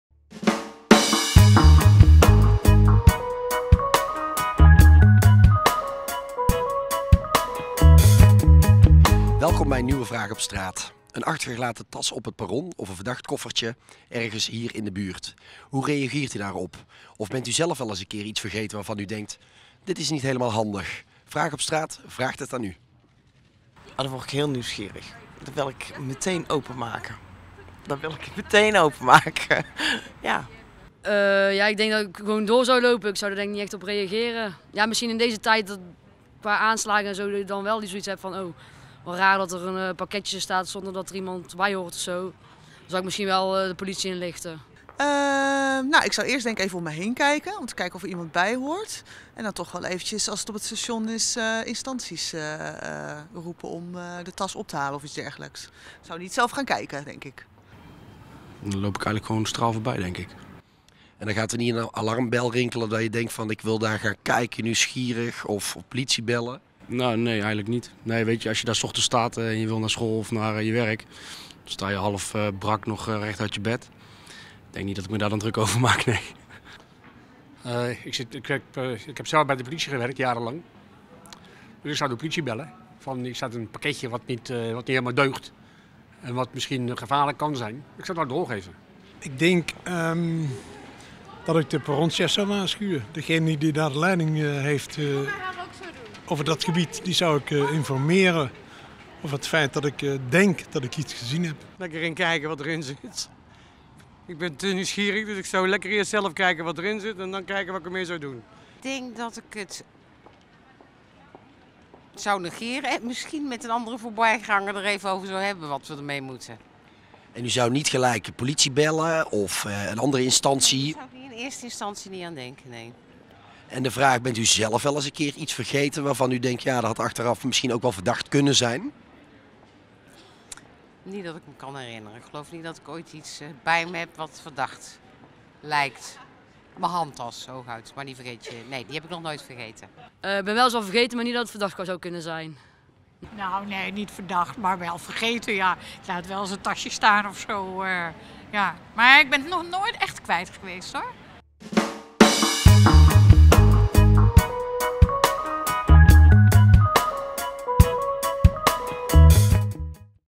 Vraag op Straat: Vreemde koffertjes
Wat zou u doen als u een mogelijk verdacht koffertje/pakketje op het perron of op straat zou zien?